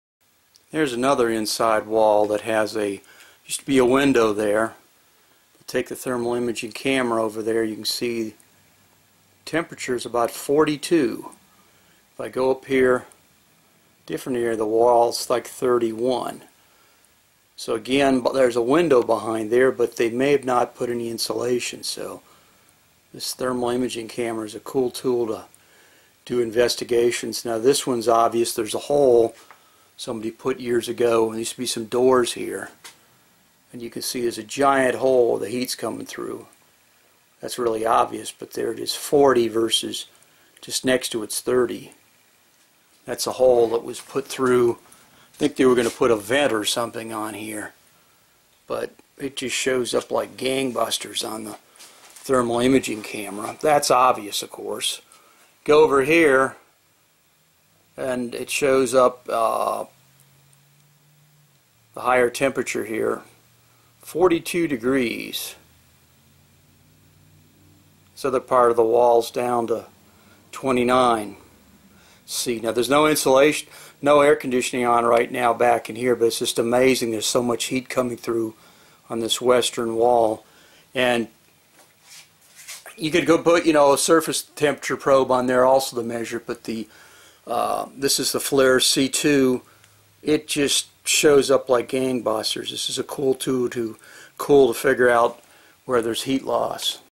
FLIR C2 Heat Loss found Poor Window blocked in Darkroom
Detecting hidden heat loss areas in a building with the FLIR C2 Thermal Imaging Camera. Room audited was a Commercial darkroom 20 years ago and never really got too cool with a 3 ton AC unit. vid0098